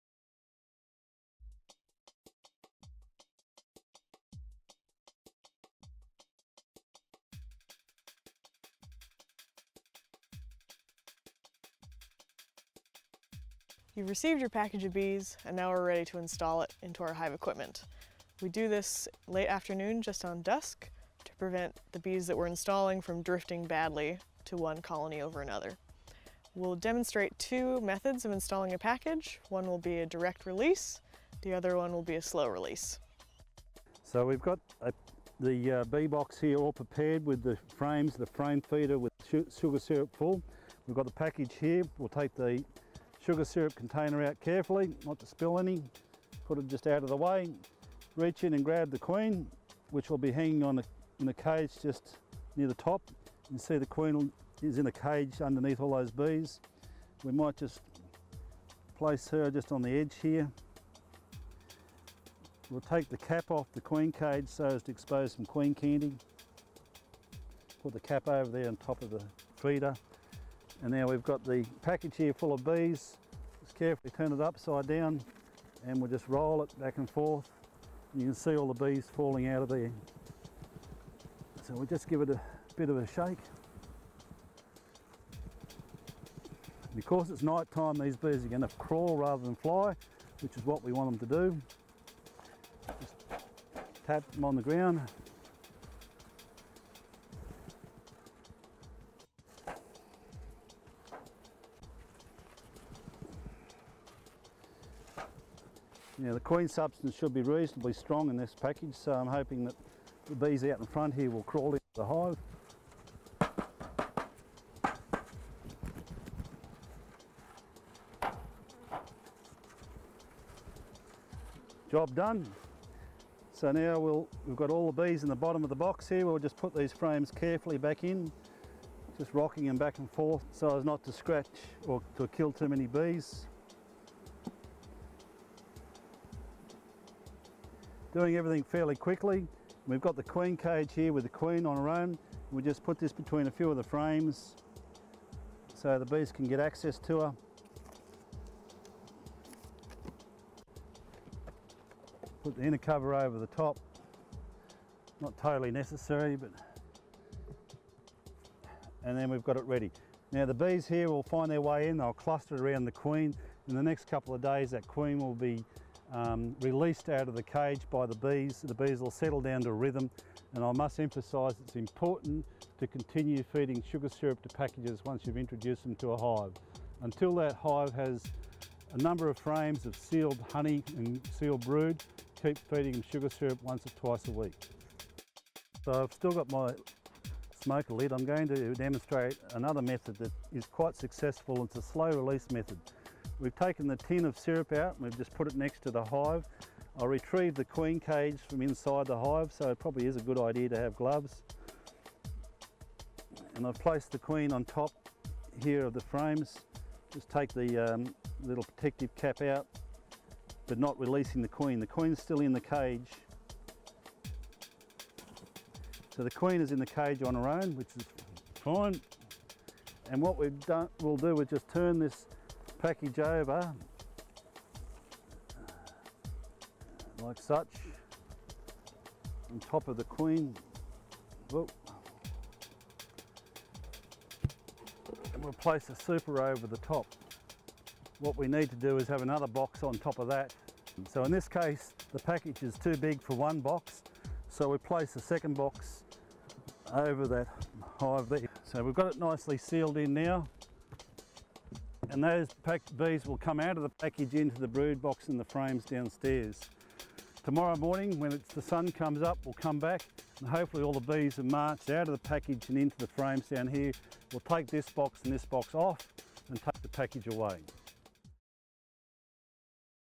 Installing Package of Bees
Doug Somerville, Technical Specialist (Bees) from the NSW Department of Primary Industries, explains how to introduce a package of bees into your brood box.